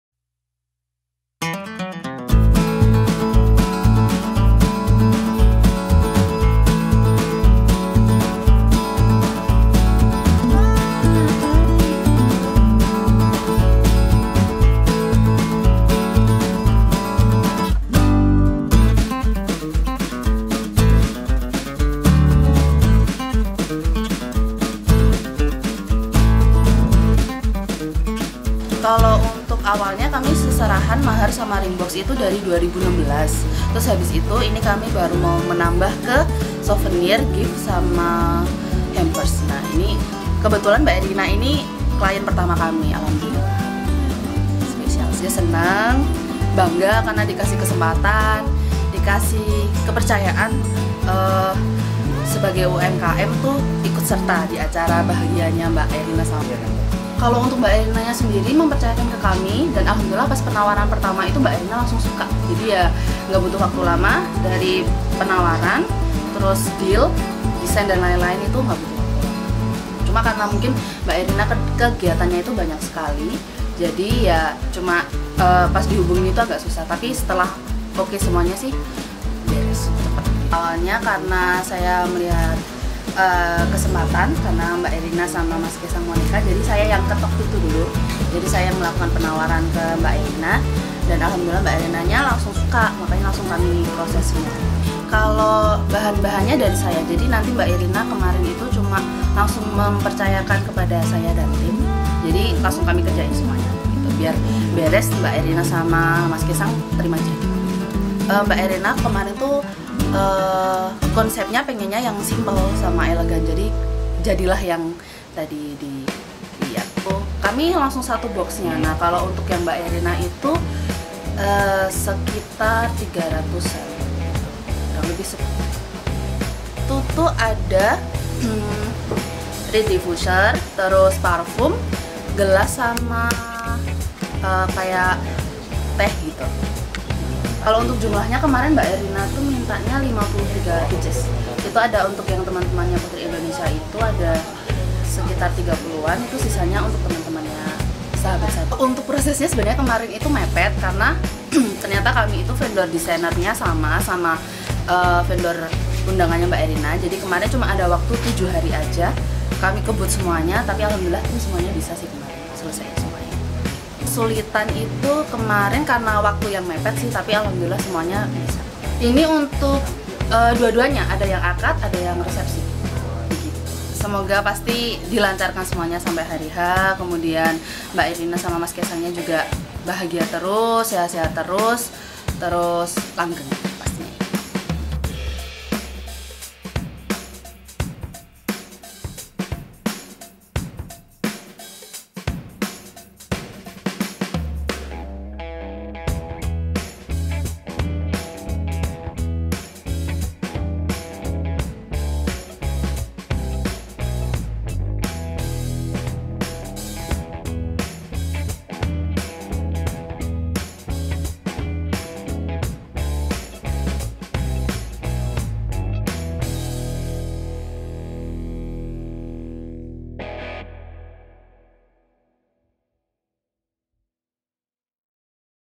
UMKM Lokal Dapat Klien Pertama Calon Mantu Presiden RI - Hampers Pernikahan Kaesang & Erina
Sekoci.net-Sebuah UMKM lokal bernama Priscanara Artelier dipercaya oleh Erina Sofia Gudono, calon istri anak bungsu Presiden Indonesia, untuk menyediakan hampers bagi sahabatnya yang akan menjadi pendamping pengantin wanita. Erina memesan sejumlah 53 paket yang diberikan kepada teman-temannya di Putri Indonesia dan sahabatnya.